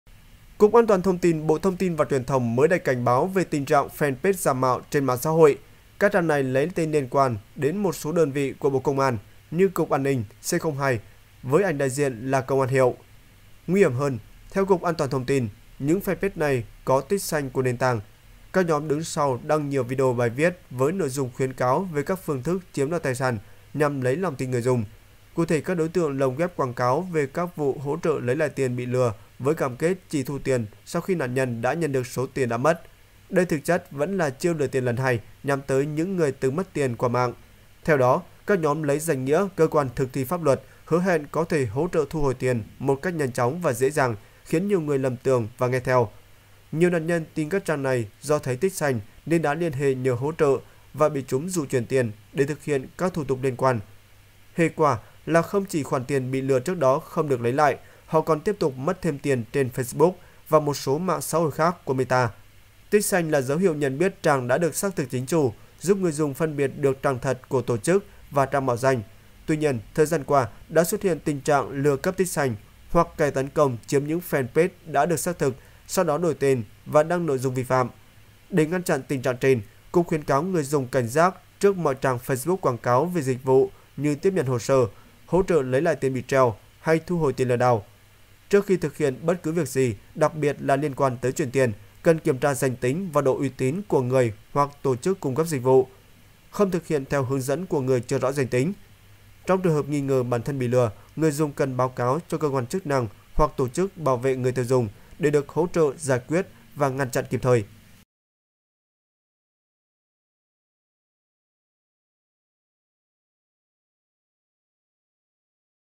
Cảnh báo thủ đoạn dùng fanpage "tích xanh" giả công an, dụ nạn nhân chuyển tiền
Cục An toàn thông tin, Bộ Thông tin và Truyền thông mới đây cảnh báo về tình trạng fanpage giả mạo trên mạng xã hội.  Các trang này lấy tên liên quan đến một số đơn vị của Bộ Công an, như "Cục An ninh", "C02" với ảnh đại diện là Công an hiệu. Nguy hiểm hơn, theo Cục An toàn thông tin, những fanpage này có tích xanh của nền tảng. Các nhóm đứng sau đăng nhiều video, bài viết với nội dung khuyến cáo về các phương thức chiếm đoạt tài sản, nhằm lấy lòng tin người dùng. Cụ thể, các đối tượng lồng ghép quảng cáo về các vụ hỗ trợ lấy lại tiền bị lừa với cam kết chỉ thu tiền sau khi nạn nhân đã nhận được số tiền đã mất. Đây thực chất vẫn là chiêu lừa tiền lần hai, nhắm tới những người từng mất tiền qua mạng. Theo đó, các nhóm lấy danh nghĩa cơ quan thực thi pháp luật, hứa hẹn có thể hỗ trợ thu hồi tiền một cách nhanh chóng và dễ dàng, khiến nhiều người lầm tưởng và nghe theo. Nhiều nạn nhân tin các trang này do thấy tích xanh nên đã liên hệ nhờ hỗ trợ và bị chúng dụ chuyển tiền "để thực hiện các thủ tục liên quan". Hệ quả là không chỉ khoản tiền bị lừa trước đó không được lấy lại, họ còn tiếp tục mất thêm tiền Trên Facebook và một số mạng xã hội khác của Meta, tích xanh là dấu hiệu nhận biết trang đã được xác thực chính chủ, giúp người dùng phân biệt được trang thật của tổ chức và trang mạo danh. Tuy nhiên thời gian qua đã xuất hiện tình trạng lừa cấp tích xanh, hoặc kẻ tấn công chiếm những fanpage đã được xác thực, sau đó đổi tên và đăng nội dung vi phạm.